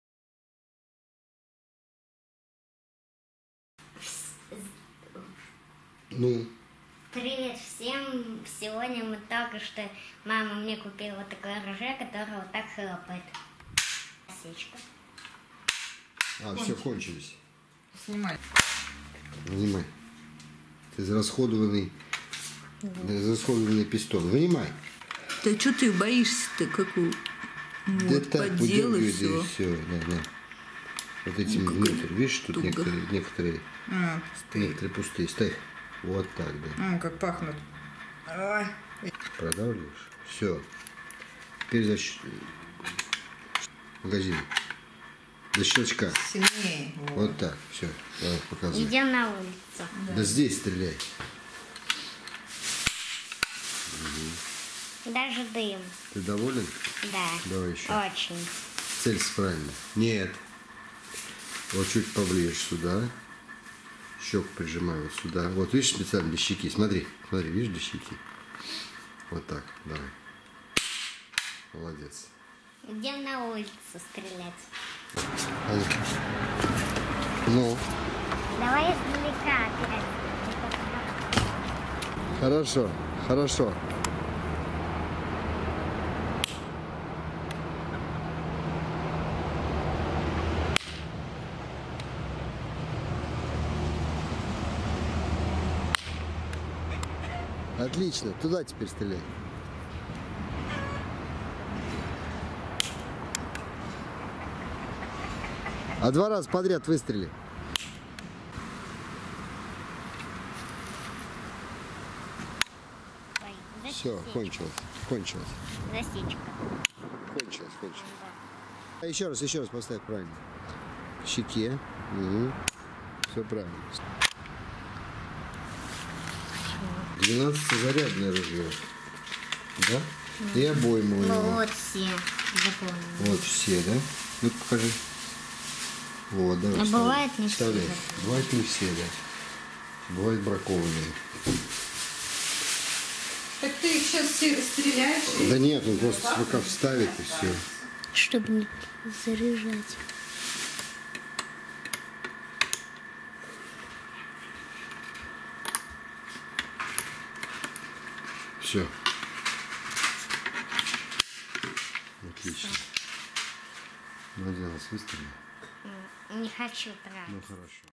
Ружьё пистонное (Испания)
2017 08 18  Детское пистонное ружье, производства Испании